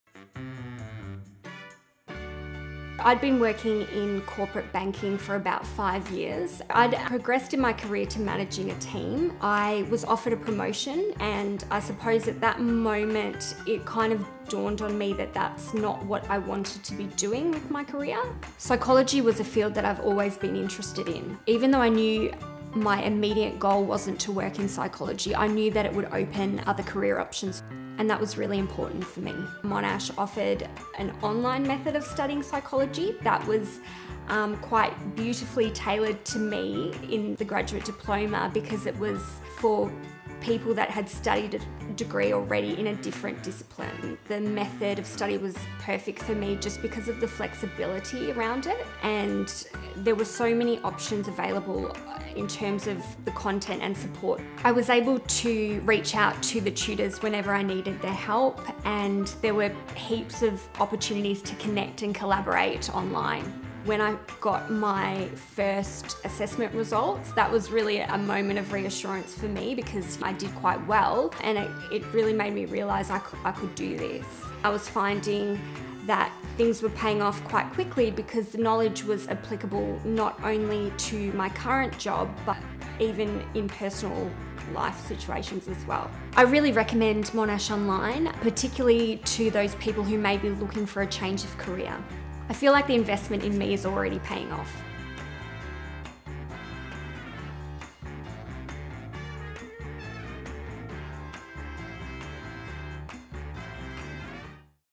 Meet Jess - Graduate Diploma in Psychology graduate
Jess' story is probably not too different from yours. After a successful career in operations at a leading financial institution, she was offered a promotion. That's when she realised she was seeking a more meaningful career. Her investment paid off and she's now using her psychology skills as a Change Manager.